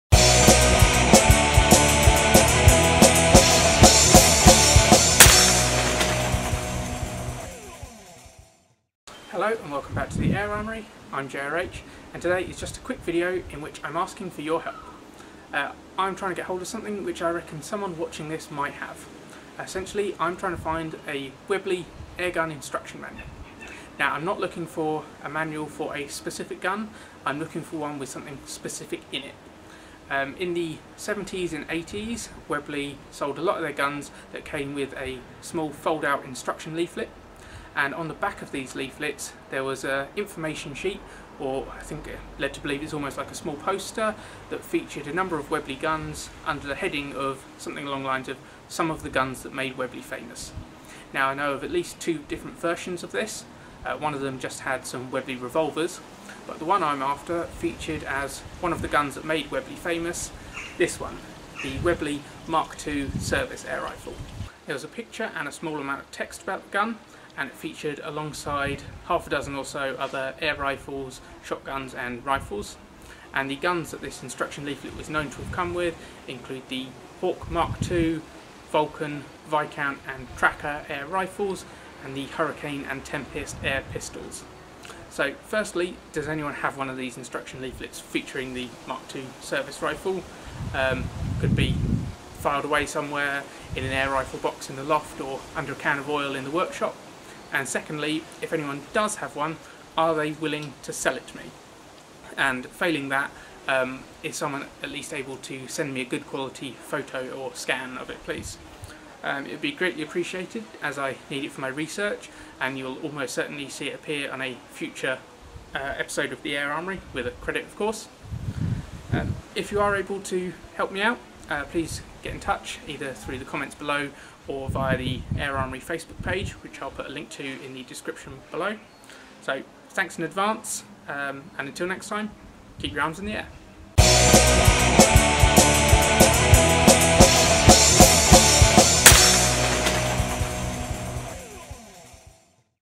Webley Airgun Instruction Manuals - I Need Your Help | Air Armoury EXTRA
Air Armoury EXTRA - Webley Airgun Instruction Manuals - I Need Your Help

The Air Armoury's JRH needs your help to track down a 1970s/1980s Webley Airgun manual/leaflet featuring some information bout the MkII Service Air Rifle.